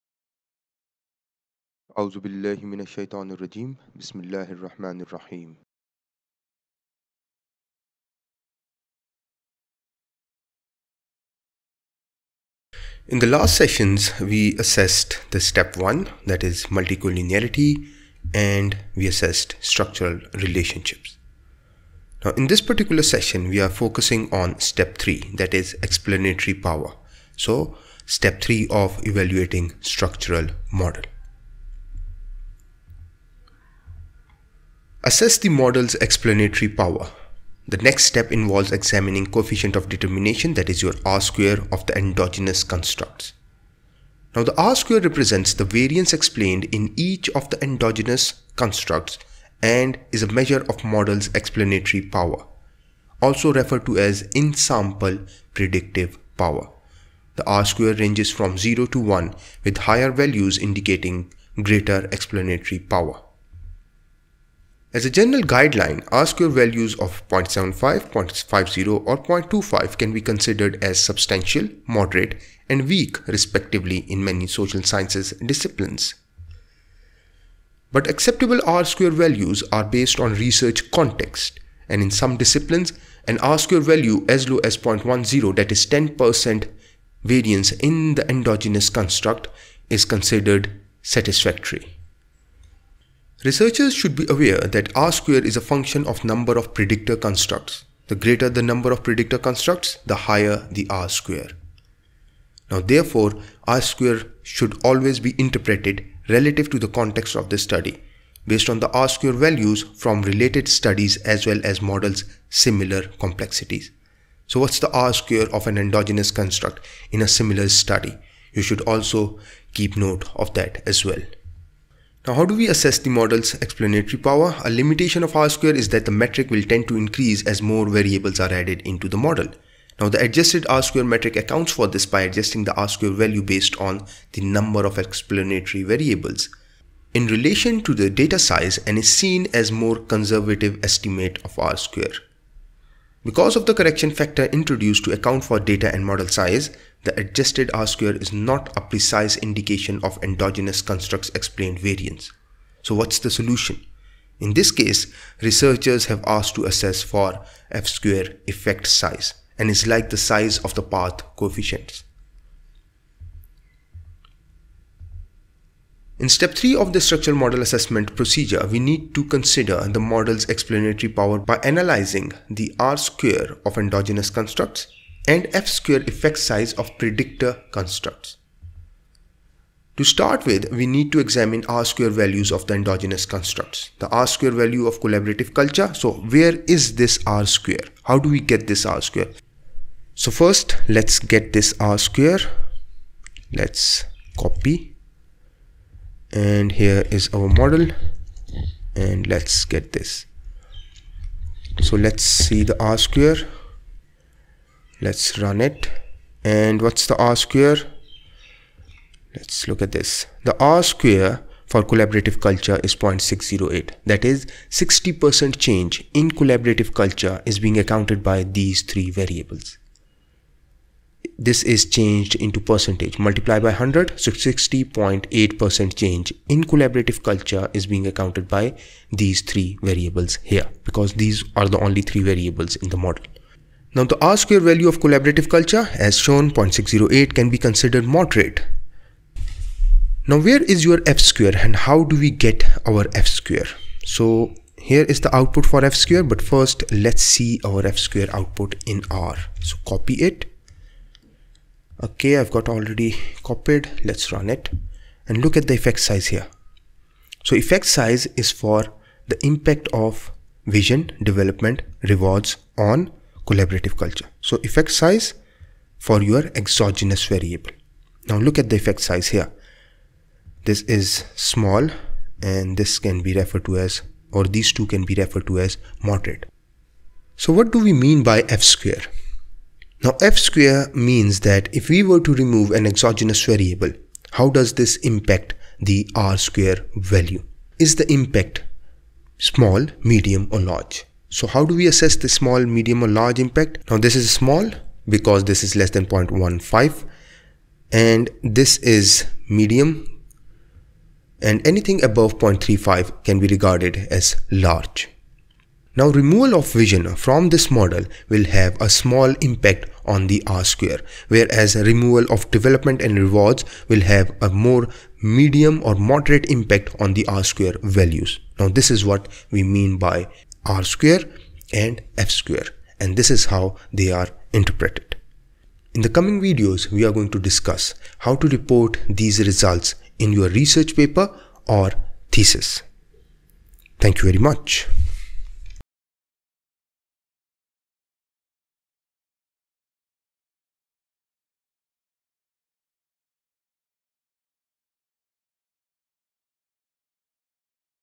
22. SEMinR Series. Evaluating Structural Model | Step 3: Explanatory Power
The focus of the session is on evaluating the structural model. Specifically, Step 2 of explaining explanatory power.